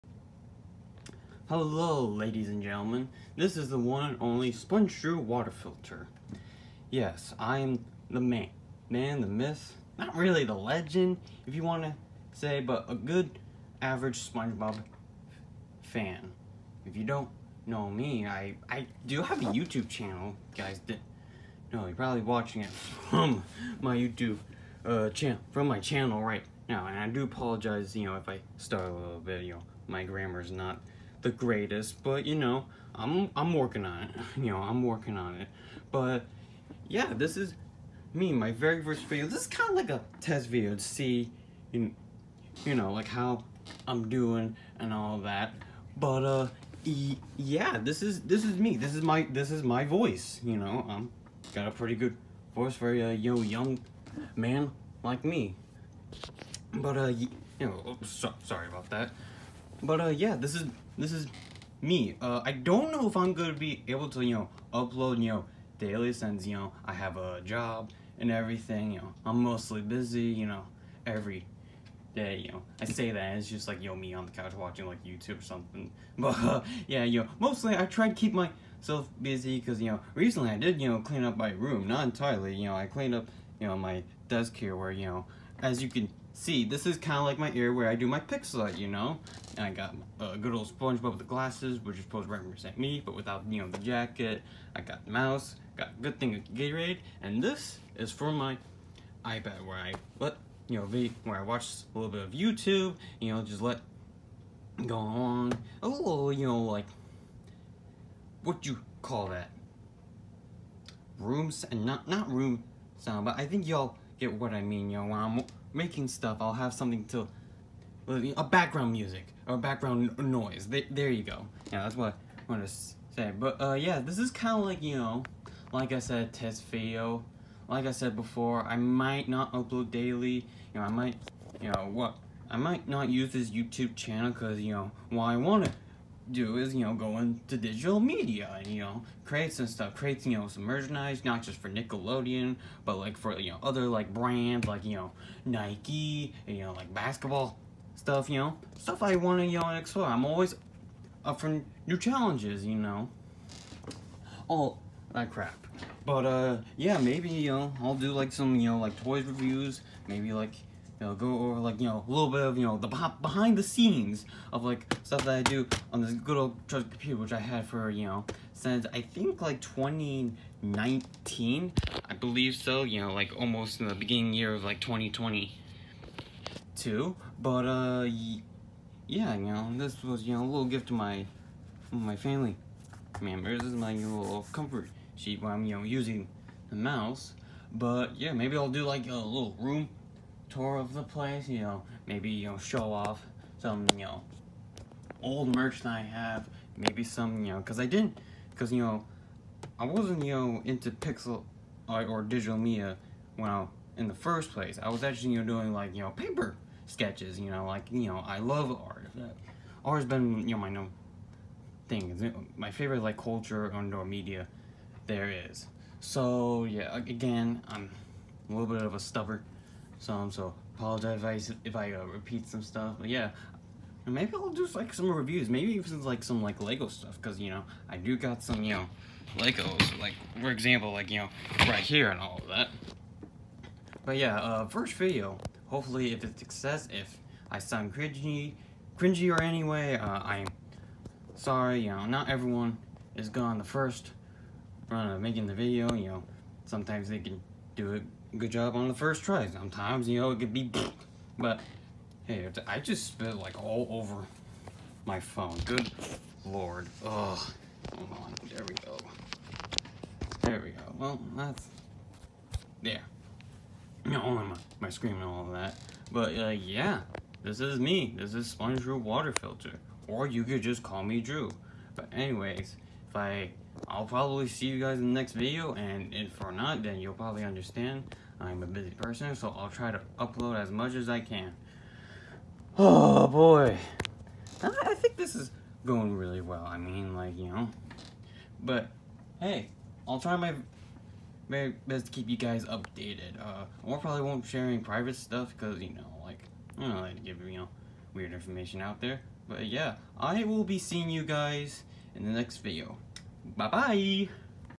My first video, obviously. 🧽
Yes, what you are hearing is the one and only SpongeDrew. Again I do apologize if this video isn’t all that great, when I said I got my computer back in 2019 almost to the beginning of 2020 I accidentally said “2022”. I have trouble in the past where my grammar gets all over the place. But yeah like I said in the video, I may not upload as much for right now. But I’ll try to upload decent content in the future, hopefully this channel will be successful in the future!